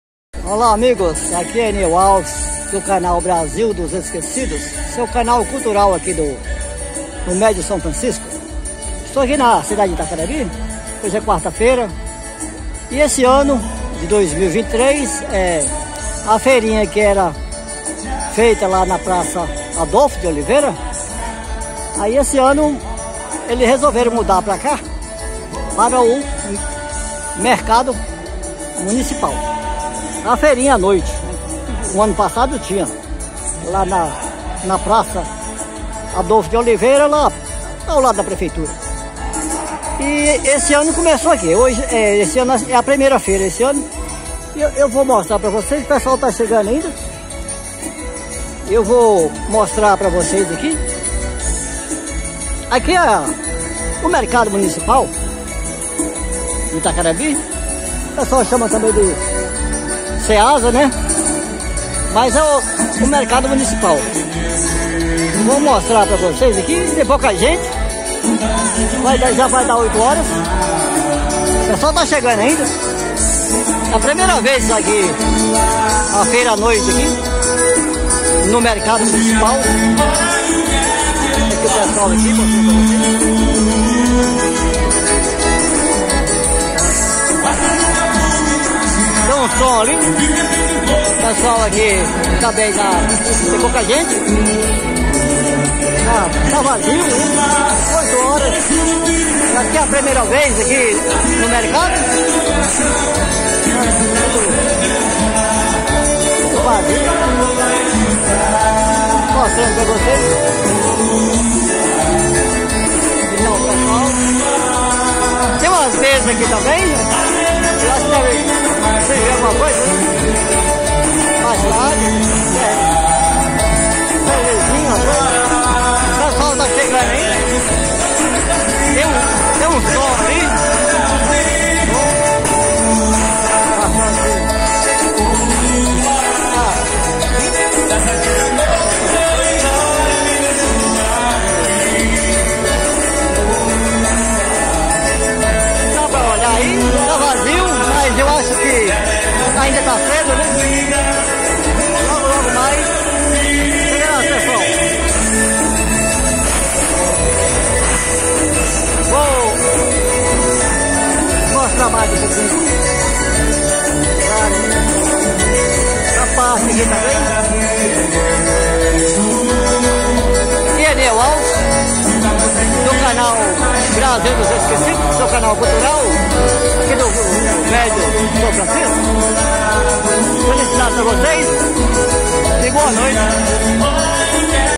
FEIRA NAS QUARTAS FEIRA Á NOITE EM ITACARAMBI NORTE DE MINAS GERAIS
Este vídeo mostra a feira nas quartas feiras á noite que foi transferida para o mercado municipal na cidade de Itacarambi norte de Minas Gerais.
Antes as feiras das quartas feiras á noite era na praça Adolfo de Oliveira ao lado da prefeitura municipal de Itacarambi MG.
Este ano foi transferida para o mercado municipal da cidade, mas parece que o povo não aprovou a mudança, pois a mesma teve pouco movimento nesta primeira feira á noite de 2023.
Esperamos que a próxima seja bem mais animada e participativa.